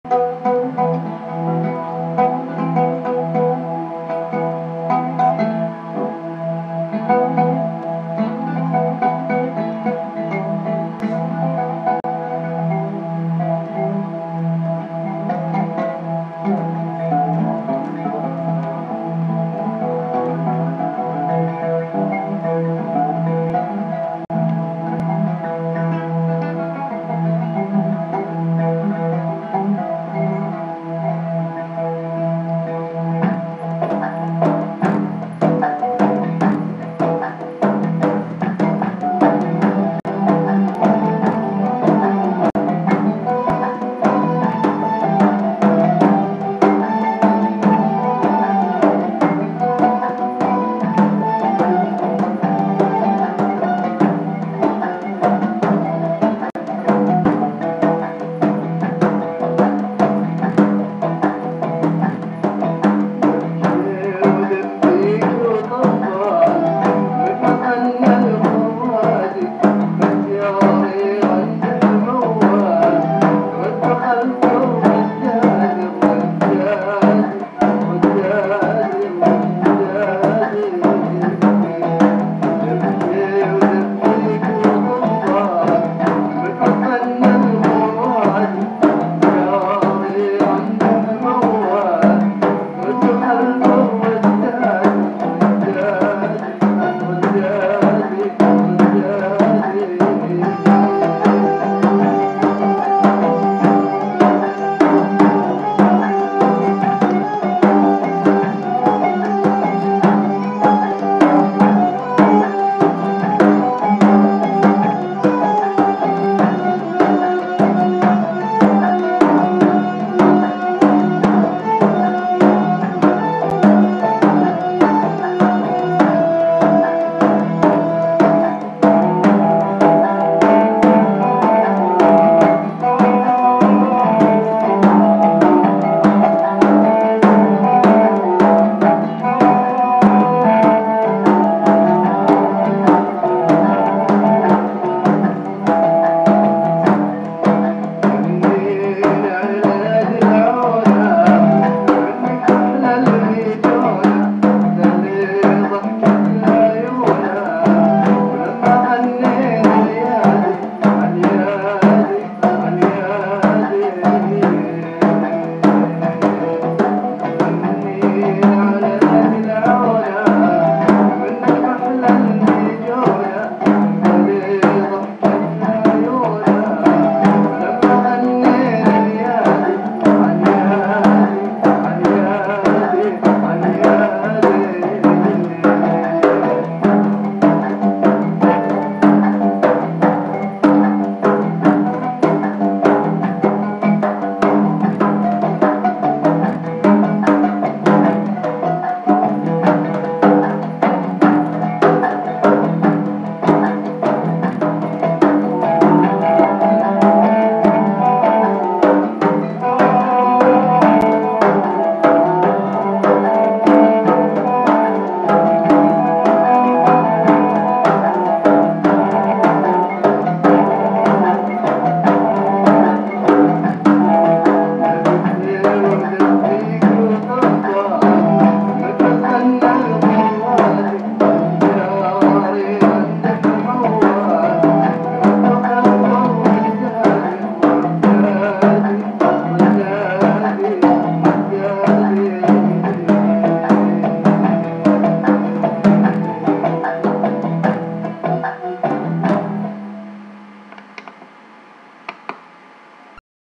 عزف دبكه ودبيكه وطبال عود + اورغ ياماها 1000
I saw this video and I liked his oud playing so much I wanted to play along with it. I rarely find something that has a really good pitch for my frame drum.